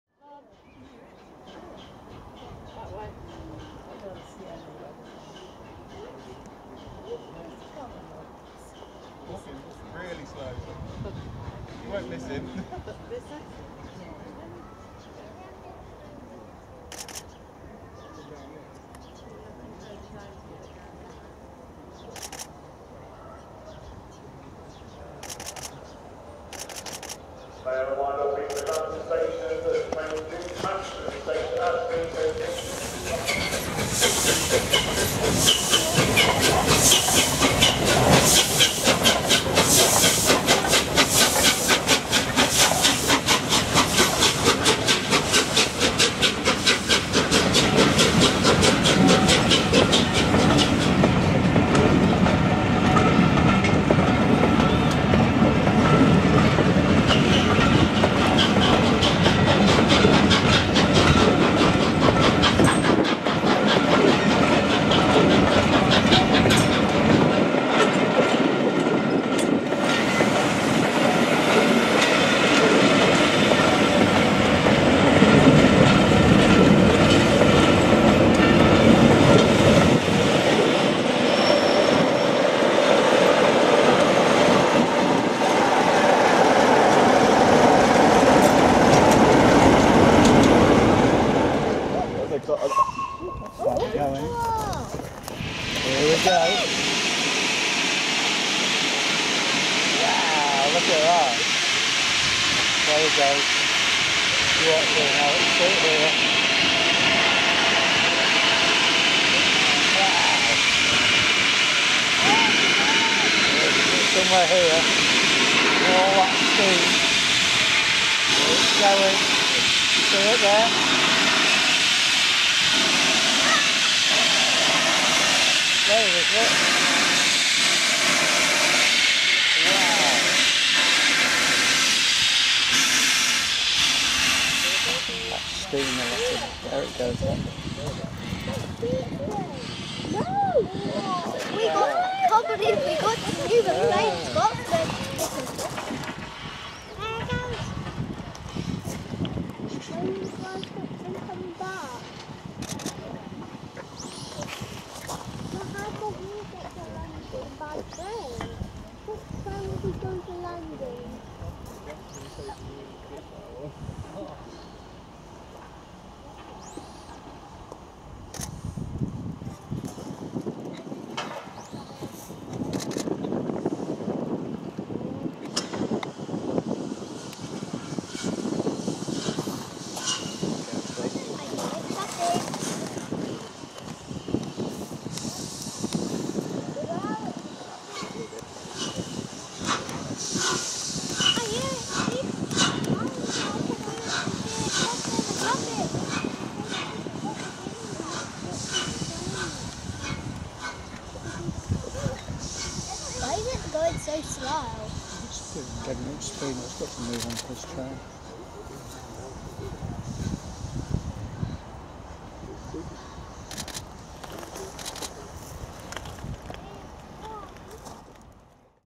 LNER (BR) 60103 'Flying Scotsman' stopping at Grantham - 25/09/21
Ex-LNER / BR locomotive 'Flying Scotsman', one of the first locomotives to go 100 mph, passing Grantham, very slowly, and stopping for replenishment.